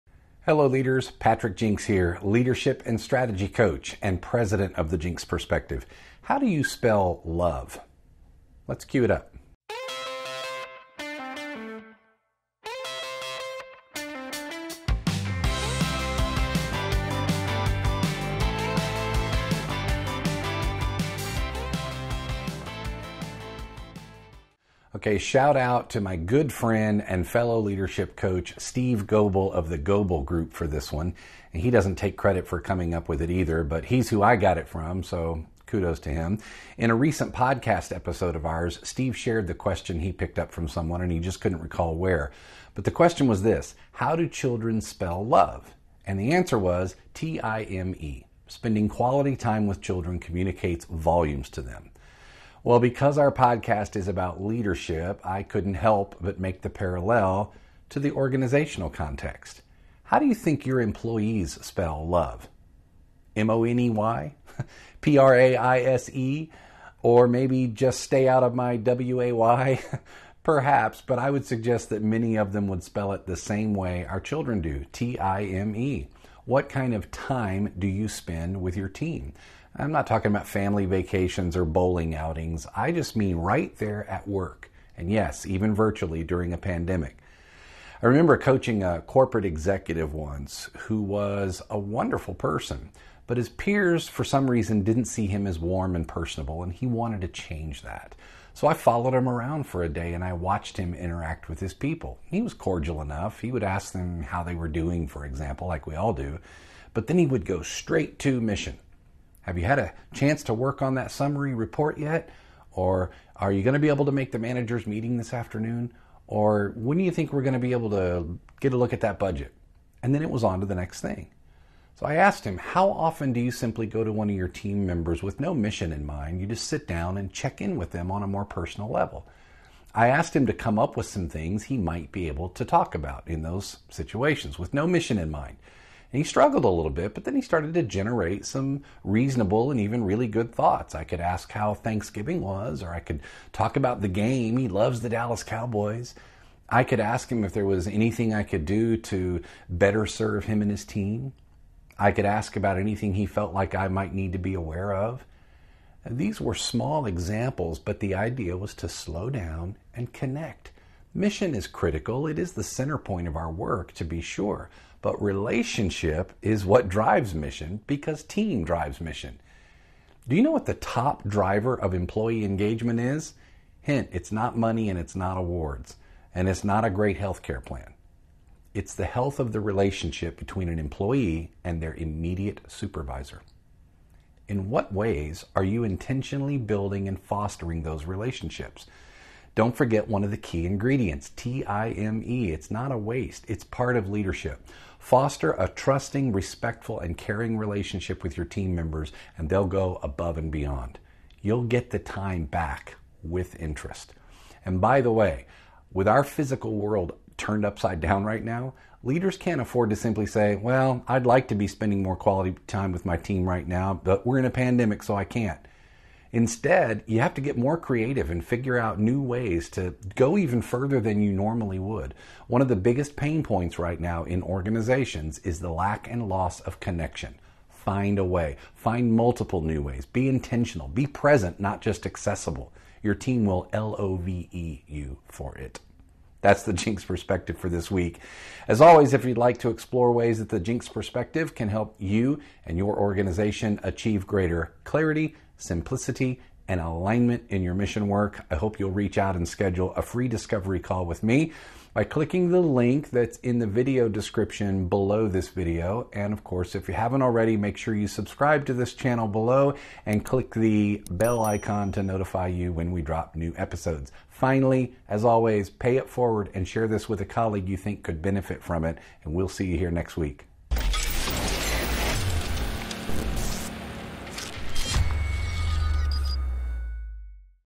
Episode 68 - How Do You Spell Love?
Patrick discusses the number one thing employees want from the leaders